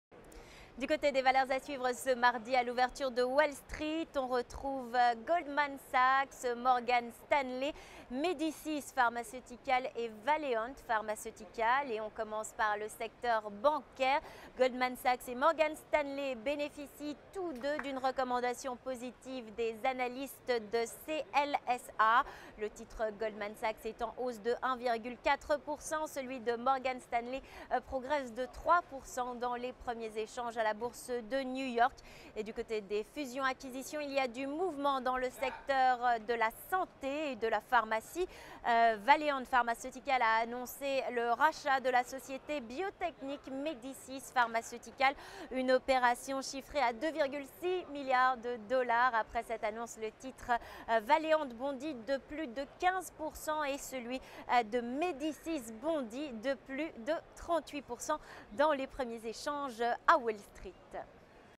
Les bancaires et pharmaceutiques en focus à Wall St.
04/09/12 Goldman Sachs et Morgan Stanley ont bénéficié de recommandations positives d'analystes; Valeant Pharmaceuticals va racheter Medicis Pharmaceuticals pour 2,6 milliards de dollars.